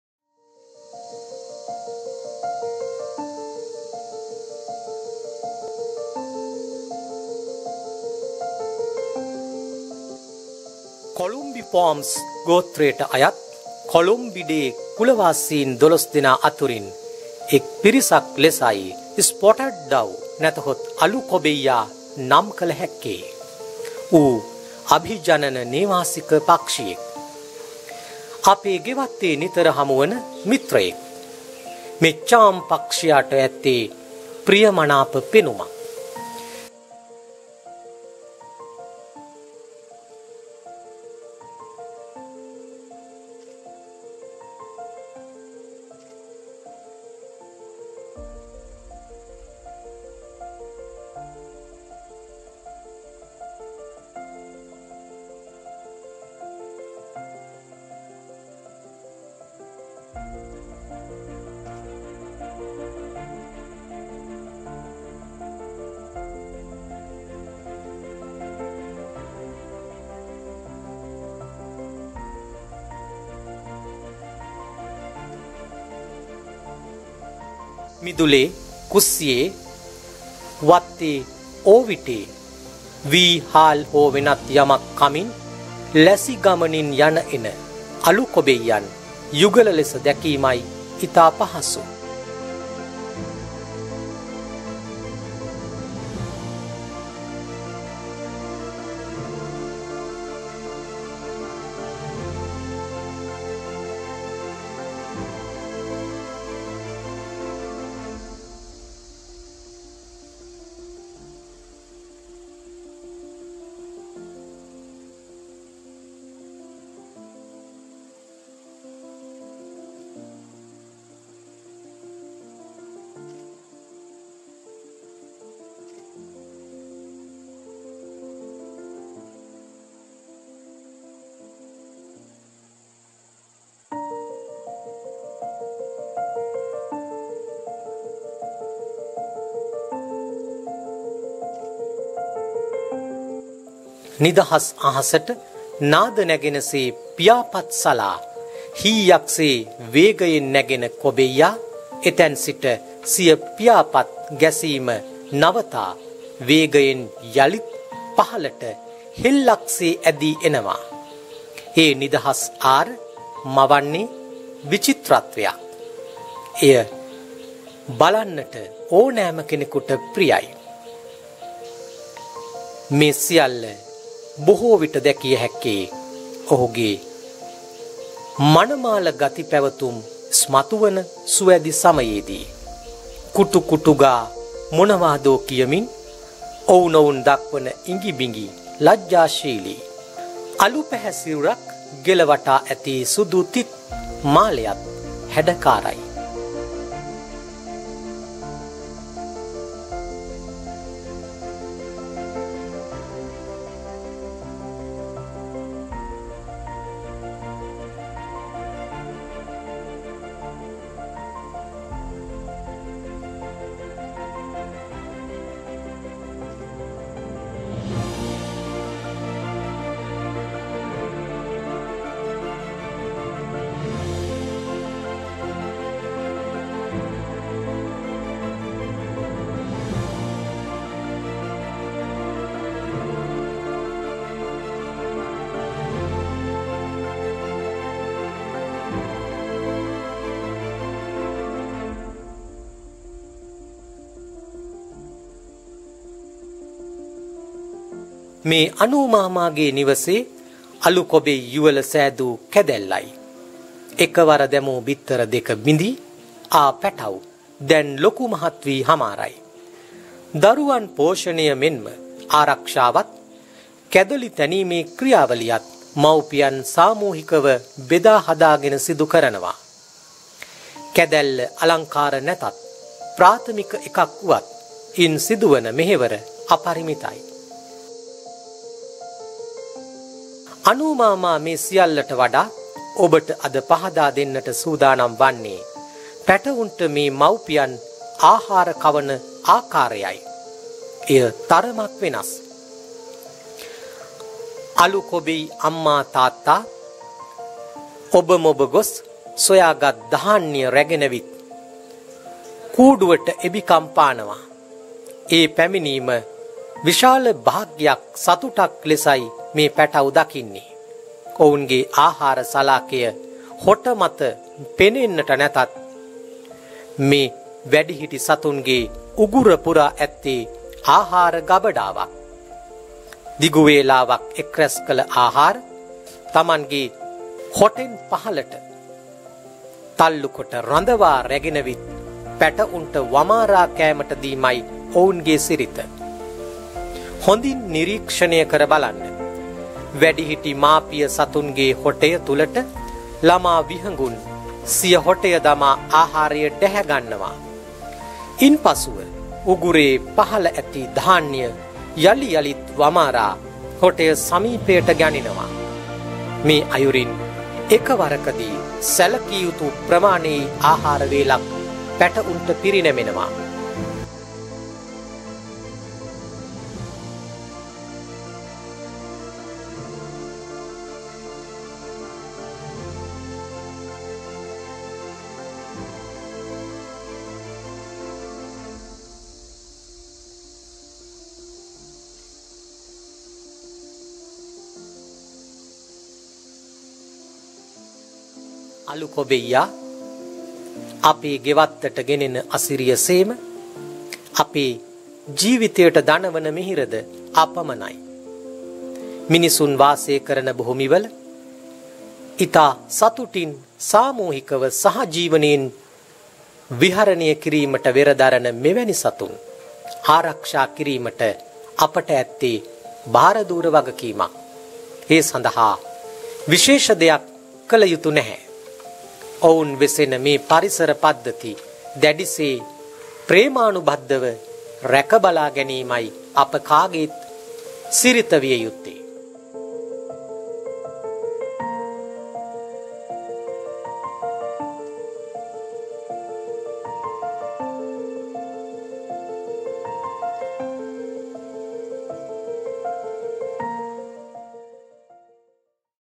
මෙහෙමත් කැවිල්ලක්..අමුතුම අළු කොබෙයියෙක්
දුවේ පුතේ, කුරුළු ලෝකය විස්මිත යි.
මේ අපට නිතර හමුවන අළු කොබෙයියන්ගේ නොපෙනෙන  ජීවිතේ  අංශුවක් පමණයි.